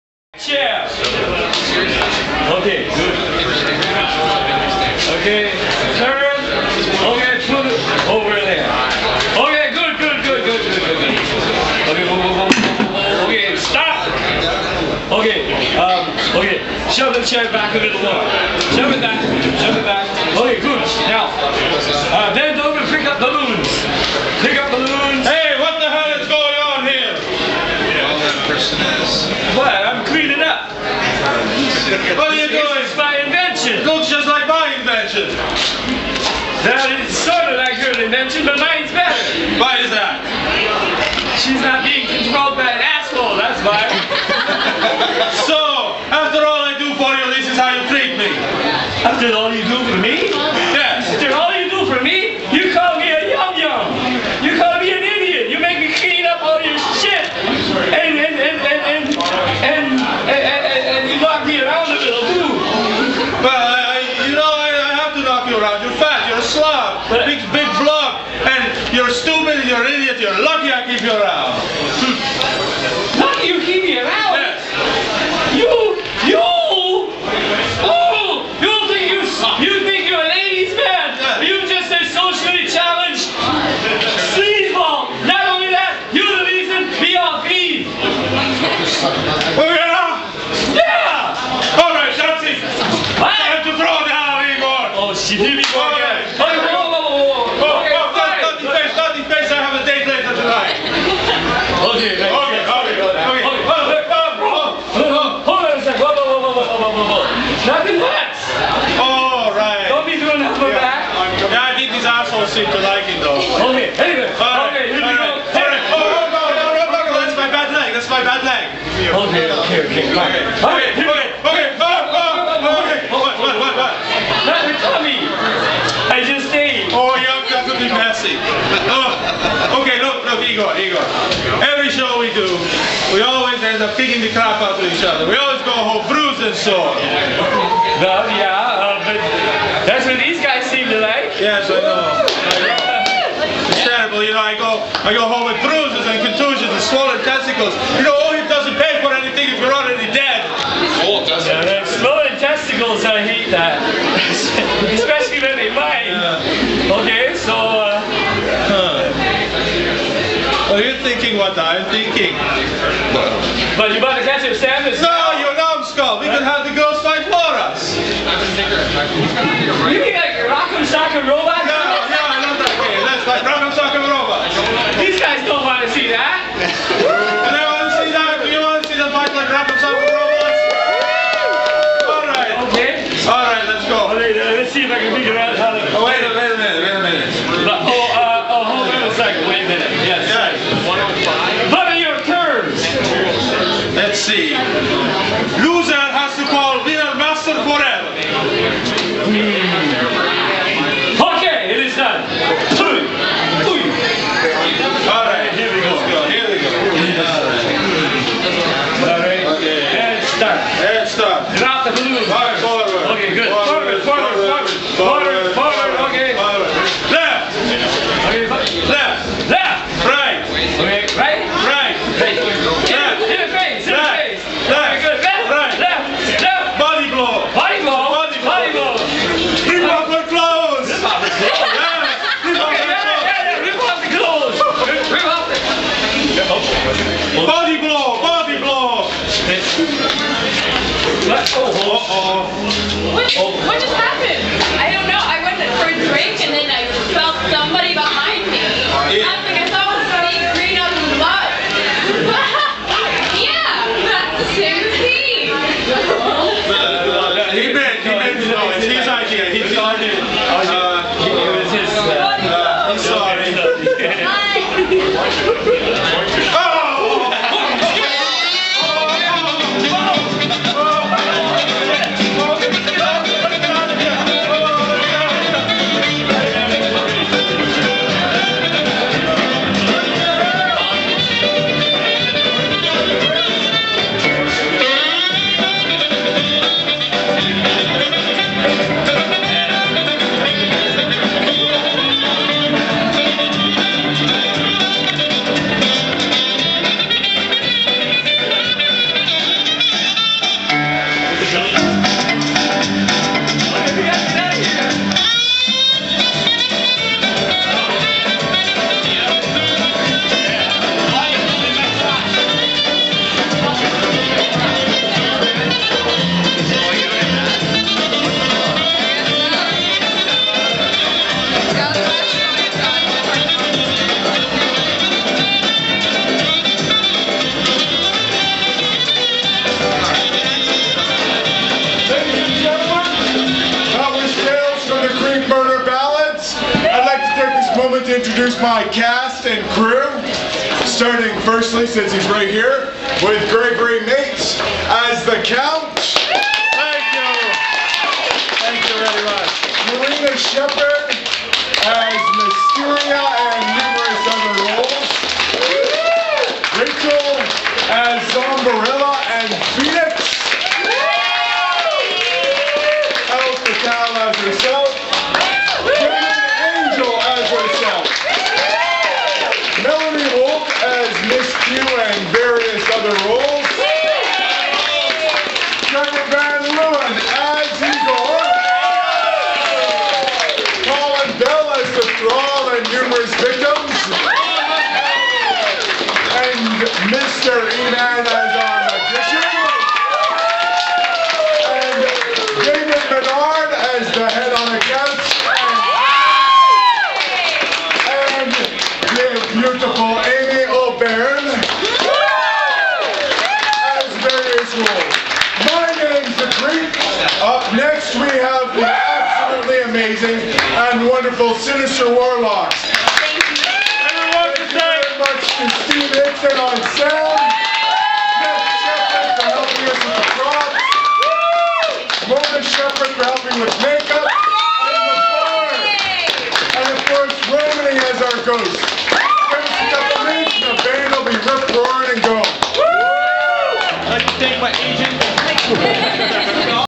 Notorious Troupe: Igor & The Count Rock 'em & Sock 'em!
On one chilly evening in London, Ontario, those lovable refugees inspired by the Hilarious House of Frightenstein, get their last fight on with the help of gorgeous Miss Cue and the dazzling Elle Fatale at Pandemic Nightclub. Notorious Troupe has a group on Facebook so you can find out about their upcoming shows.